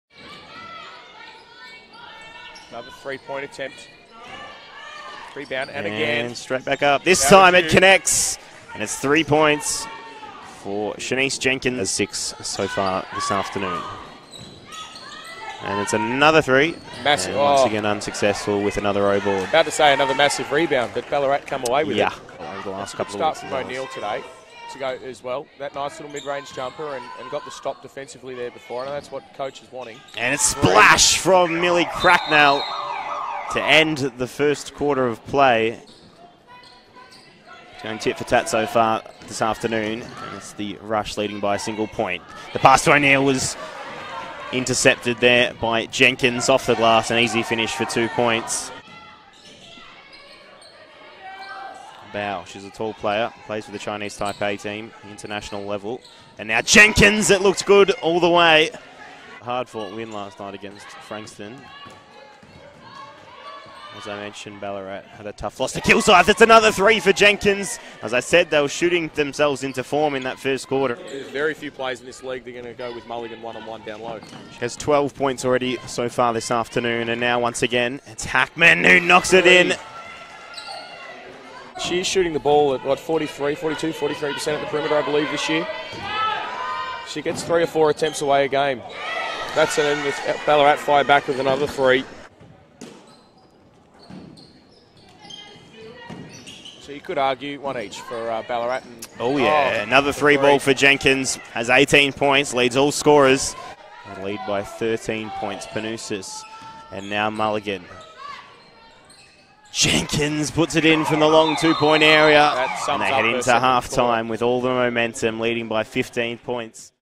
Chanise Jenkins (20 points) Highlights vs. Eltham
Chanise Jenkins (20 points) Highlights vs. Eltham, 05/12/2019